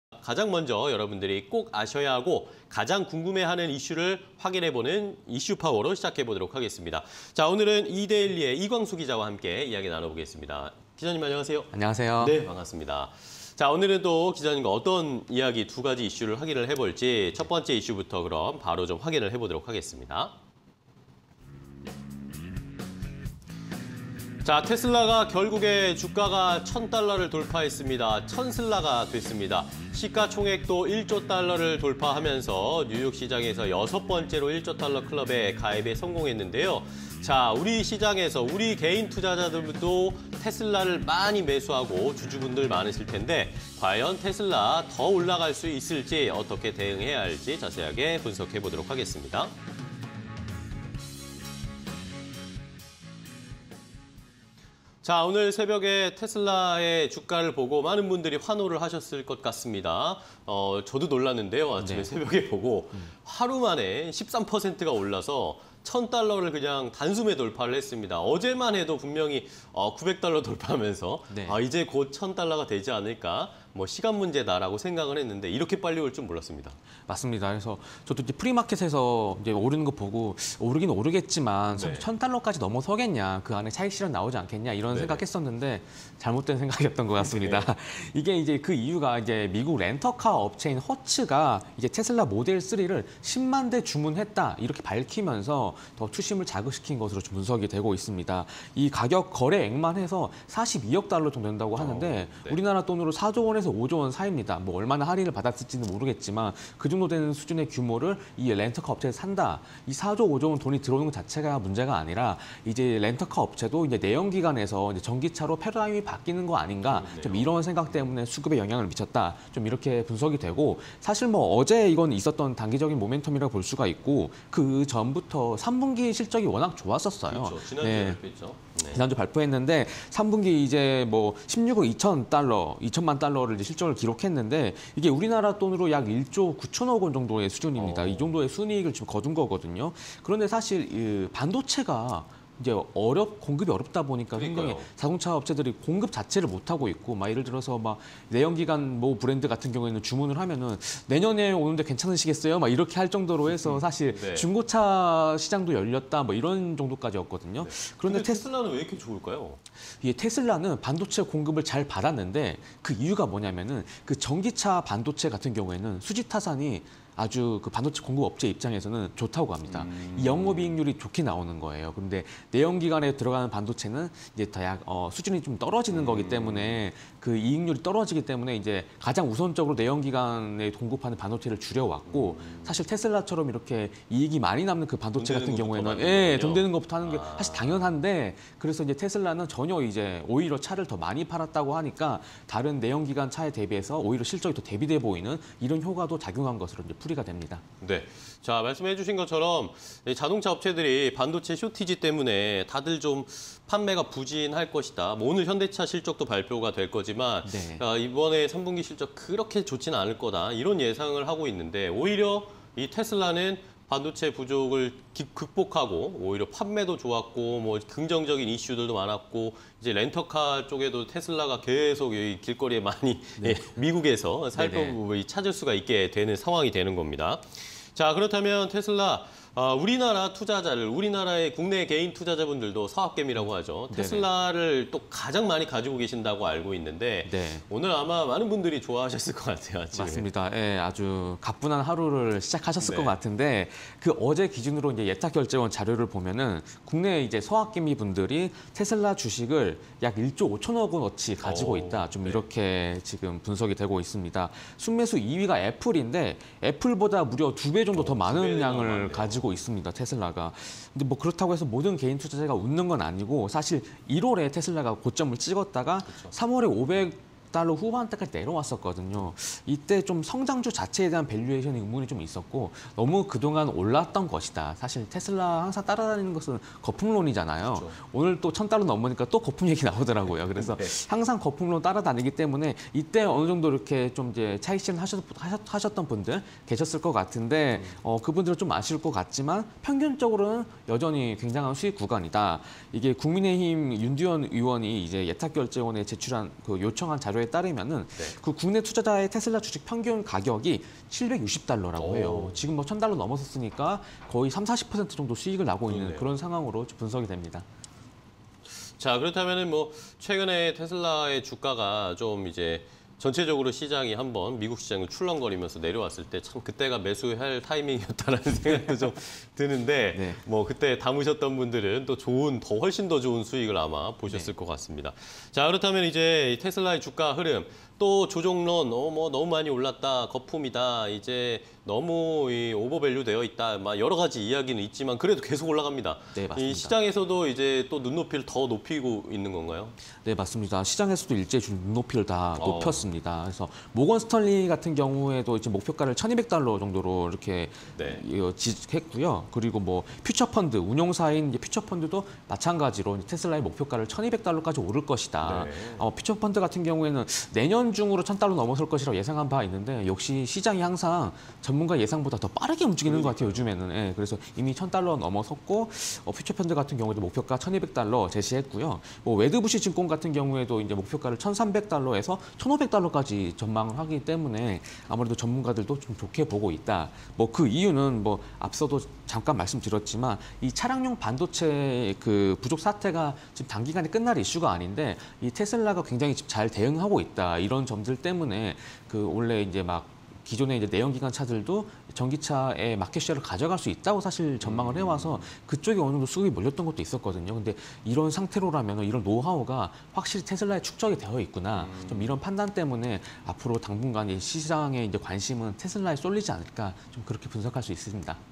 '천슬라' 됐다… 테슬라, 시총 1조 달러 돌파_이슈 파워 (20211026)
Q. '테슬라'의 거침없는 '독주'… 원인은?
- 테슬라, 전 세계 자동차 업체 최초 1조 달러 클럽 가입
- 렌터카 업체 허츠, '모델3' 10만 대 구매 계획
- 올해 3분기, 사상 최대 실적 기록… 전년비 5배 이상 성장
- 차량용 반도체 부족… 대체 칩 공급으로 대응
- 블룸버그 "반도체 쇼티지 사태, 전통적 車 업체보다 대처 잘해"

Q. 서학 개미 '아뿔싸!', 테슬라 얼마나 담았나?
- '서학 개미' 테슬라 매수 규모, 애플 대비 약 2배 수준
- 테슬라 매수한 '서학 개미' 평균 약 20% 수익권

Q. 테슬라, '천슬라' 돌파… 전문가 분석은?
- 美 증권가, 테슬라 수익ㆍ주가 전망치 상향 조정
- 모건 스탠리, 목표 주가 900달러 → 1200달러 상향 조정 영향도
- 美 증권사 웨드부시, 테슬라 목표가 1,500달러로 상향